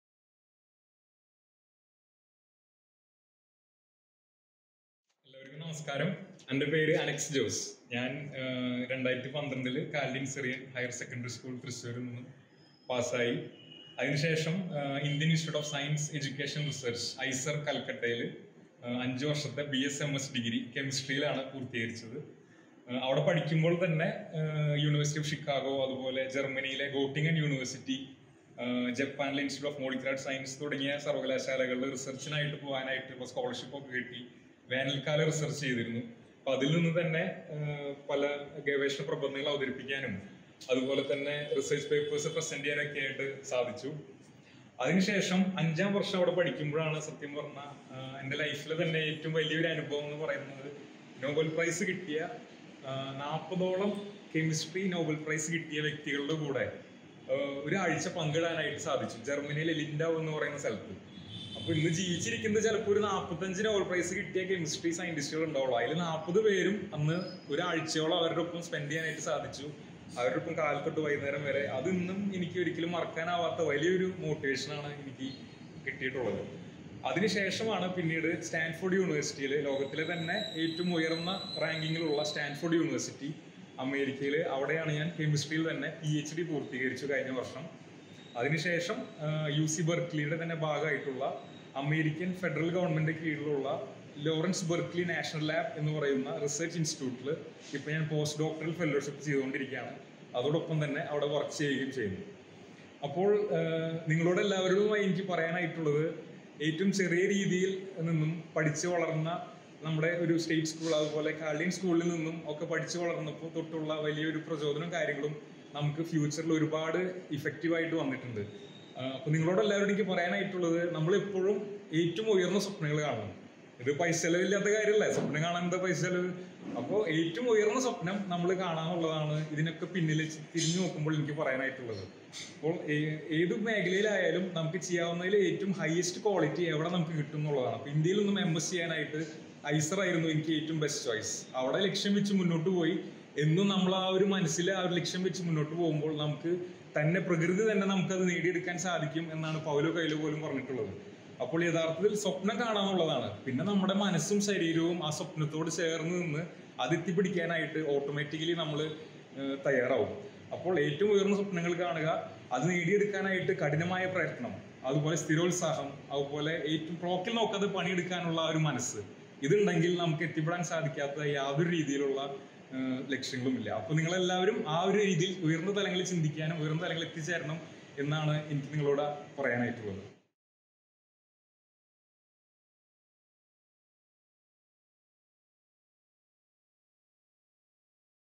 Talk with Dr. Anex Jose | Part 1
Join us in this powerful conversation with Dr. Anex Jose as he shares his incredible journey from the classrooms of Kerala to the prestigious laboratories of Silicon Valley! 🚀
In this candid talk, Dr. Anex opens up about his remarkable academic odyssey - from completing his schooling in God's Own Country to pursuing BS-MS at the renowned IISER Kolkata, then making the leap to Stanford University for his PhD. His journey didn't stop there - with enriching experiences at Göttingen University, Germany, and IMS Japan, he now contributes to groundbreaking research at the world-famous Lawrence Berkeley National Laboratory.

Whether you're a student planning your future, a researcher seeking inspiration, or someone who believes in the power of dreams, this conversation will ignite something within you! 🔥